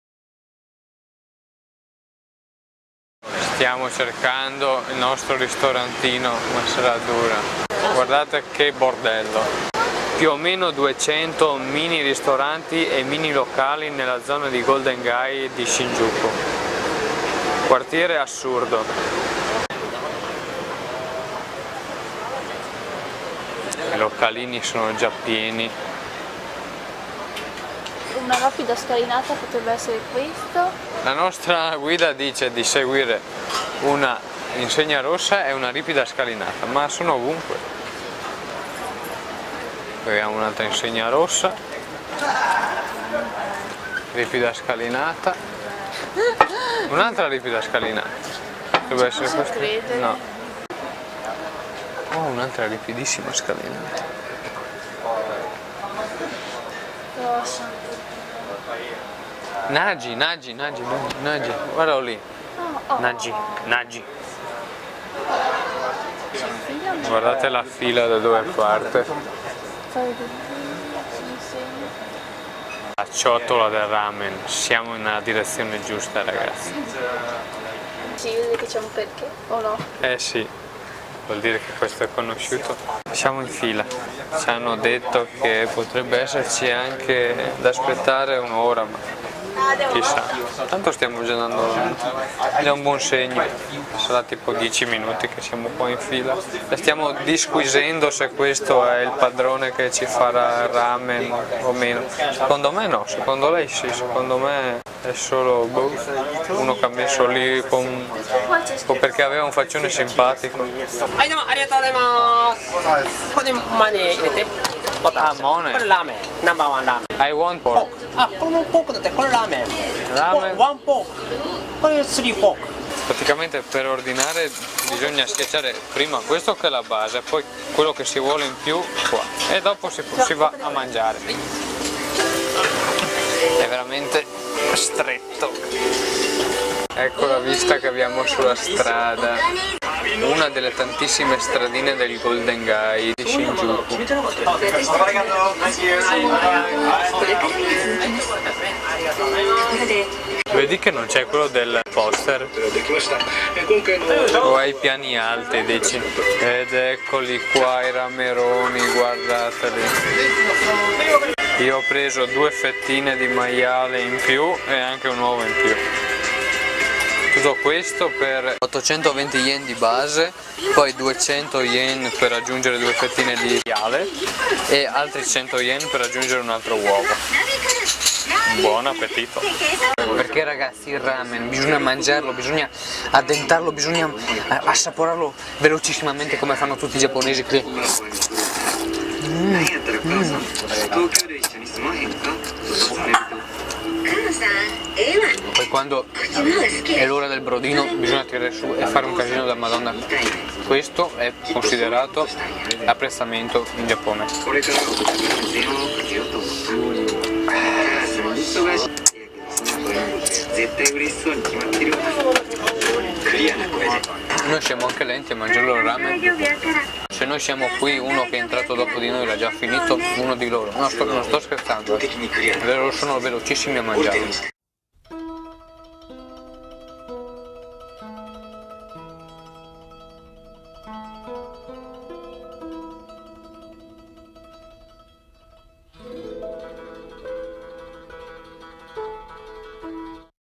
TOKYO VLOGS #26 - Ramen Nel Golden Gai Di Shinjuku (ITA - ENGLISH SUBS)
ITALIAN Vlog with English Subtitles

ITALIANO
3° Parte del Vlog del 11/10/2014

Ritorno a Tokyo! E lo facciamo andando dritti dritti a Shinjuku, il quartiere che non dorme mai.
Ma la fame chiama e prima di visitare il quartiere ci fiondiamo in una particolare zona chiamata Golden Gai: Una serie di strettissime vie ripiena di piccolissimi locali e ristoranti...solo qui se ne contano oltre 200!
La nostra ricerca ci porta da "Nagi", un locale di Ramen che a giudicare dalla fila fuori si mangia un gran bene! ;)
Ma soprattutto, seguite i preziosi consigli di Valerio su come si deve mangiare Ramen in un vero jap style e non farete mai brutta figura! ;)

ENGLISH
3rd part of the Vlog of 11/10/2014

Back in Tokyo! We go straight in Shinjuku, the neighborhood that never sleep.
But the hunger calls and before to see the neighborhood we dash in a characteristic area called Golden Gai: a series of very tight street full of small bars and restaurants.. Only in this area we count more 200 of them.
Our journey bring us to "Nagi", a ramen restaurant in which, judging by queue outdoor, the food is very good! ;)
But above all, follow Valerio's valuable advices about eating ramen in a truly jap-style and you will never make a bad impression! ;)